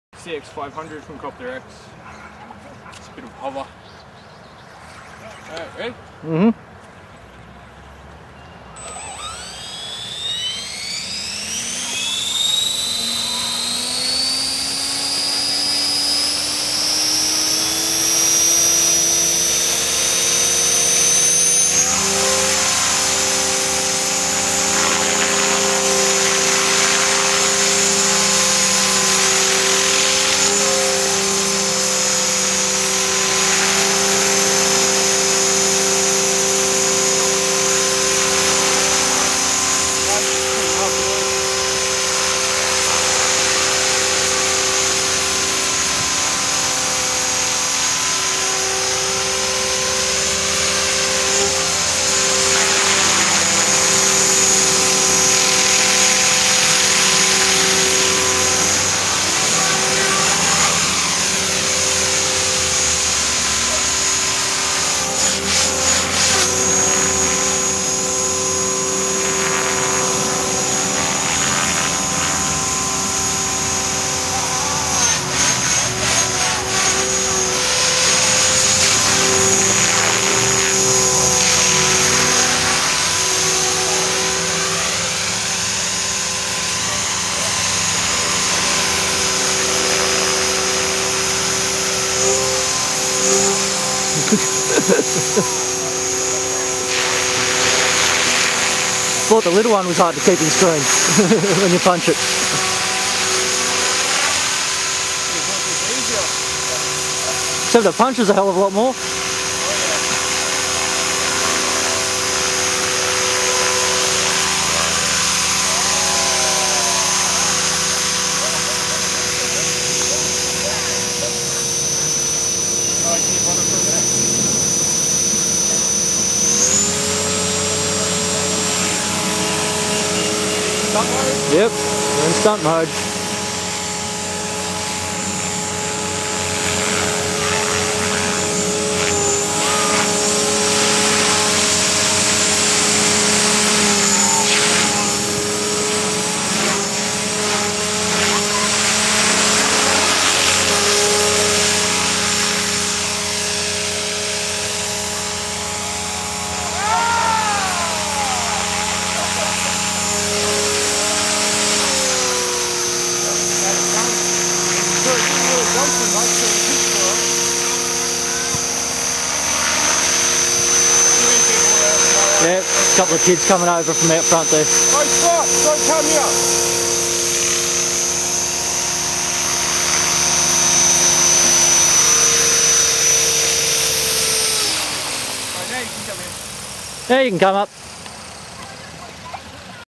CopterX CX500
First hover / setup test of my CX500.
The motor & ESC is the kit gear, 1600kv motor, running a 6s Reactive lipo pack, Dx6i using duel band (two receivers), HS-5245MG's for cyclic & Hitech HS5000 Gyro with a 9257 servo. Still need to adjust & setup the tail a little better & double check tracking. At take off I am in normal mode with a max throttle of 80% at the top of the curve, about 60~70% at take off. I had set stunt mode at 80% flat - after this test it will go to 90 ~ 100% either flat or curve.